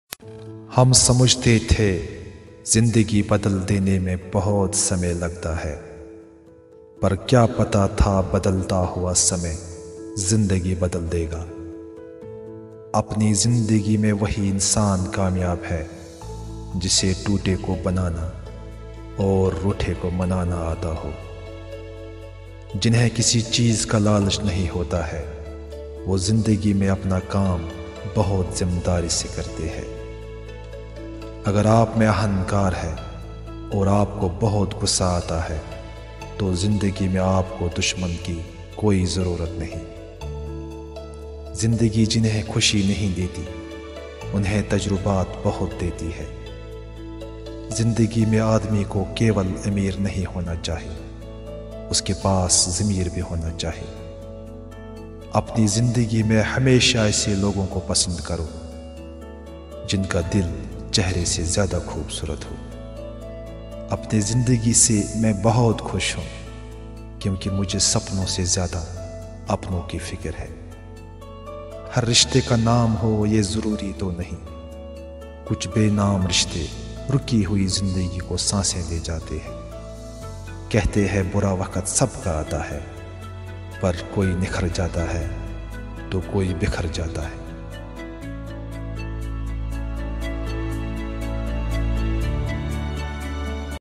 zindagi se darte ho|beautiful life quotes
1.हम सोचते थे ज़िन्दगी बदलने में बहुत समय

      लगेगा !
      पर क्या पता था बदलता हुआ समय ज़िन्दगी
      बदल देगा !!"

2. "अपनी ज़िन्दगी में वही इंसान कामयाब है !
      जिसे टूटे को बनाना और रूठें को मनाना आता
      हैं !!"

3. "जिन्हें किसी चीज़ का लालच नहीं होता है !
      वो ज़िन्दगी में अपना काम बहुत ज़िम्मेदारी से

      करते हैं !!"

4. "अगर आपमें अहंकार है और आपको बहुत गुस्सा
      आता है !
      तो ज़िन्दगी में आपको किसी और दुश्मन की कोई
      ज़रूरत नहीं !!"

5. "ज़िन्दगी जिन्हें खुशी नहीं देती !
      उन्हें तजुर्बे बहुत देती है !!"

6. "ज़िन्दगी में आदमी को केवल अमीर नहीं होना
      चाहिए !
      उसके पास ज़मीर भी होना चाहिए !!"

7. "अपनी ज़िन्दगी में हमेशा ऐसे लोगों को पसंद
      करो !
      जिनका दिल चेहरे से ज़्यादा खूबसूरत हो !!"

8. "अपनी ज़िन्दगी से मैं बहुत ख़ुश हूँ !
      क्योंकि मुझे सपनो से ज़्यादा अपनों की फ़िक्र
       है !!"

9. "हर रिश्ते का नाम हो ये ज़रूरी तो नहीं !
      कुछ बेनाम रिश्ते रुकी हुई ज़िन्दगी को सांसे दें
      जाते हैं !!"